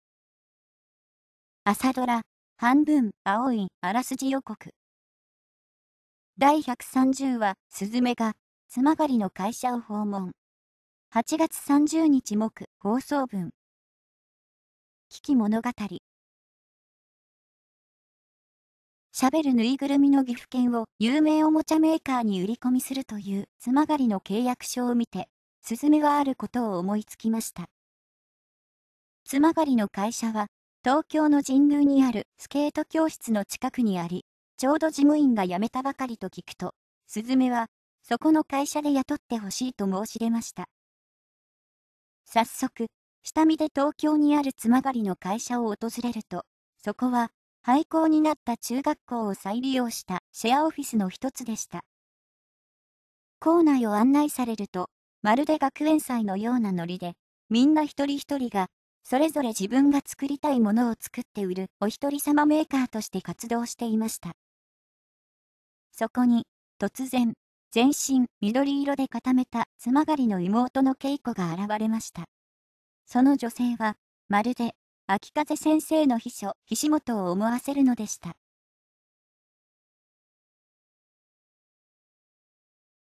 朝ドラ「半分、青い。」あらすじ予告 第130話 8月30日（木）放送分 －聴きものがたり
朝ドラ「半分、青い。」あらすじ予告 第130話 鈴愛が津曲の会社を訪問 8月30日（木）放送分 －聴きものがたり